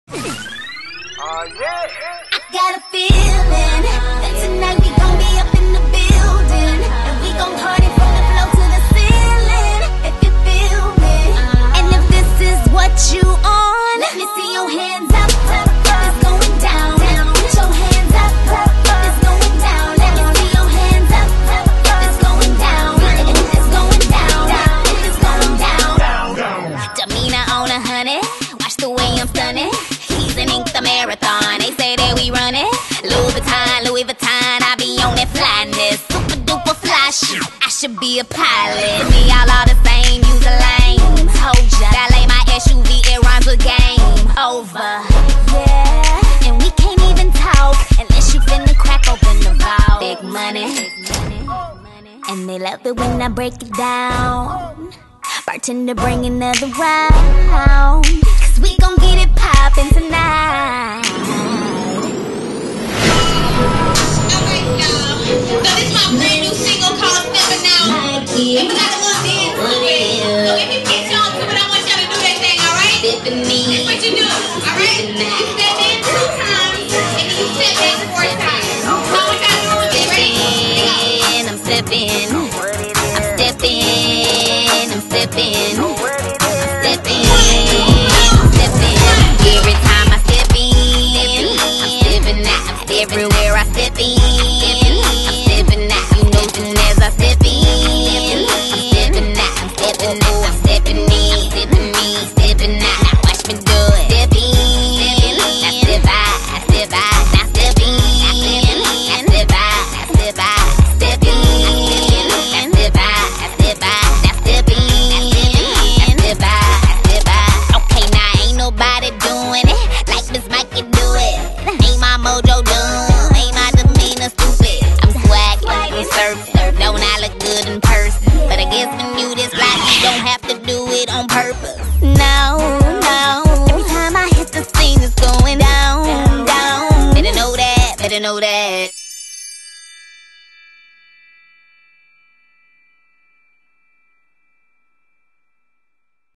Miss Mykie: Live Performance @ NYLO (Dallas, Tx.)
Live in Plano, Tx (North Dallas) for a Sweet 16 Part @ NYLO Hotel.